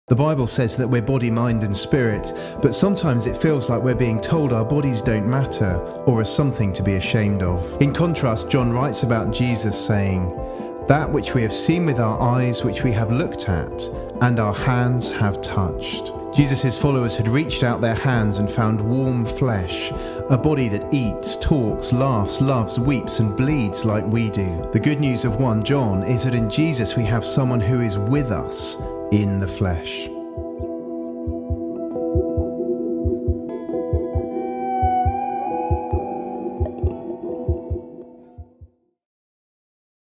In The Flesh
1 // Where do people hear the message that their body is something to be ashamed of?
2 // How does the affirmation of Jesus' body affect the way your ink about your own body?

Spoken & Written by Jon Marlow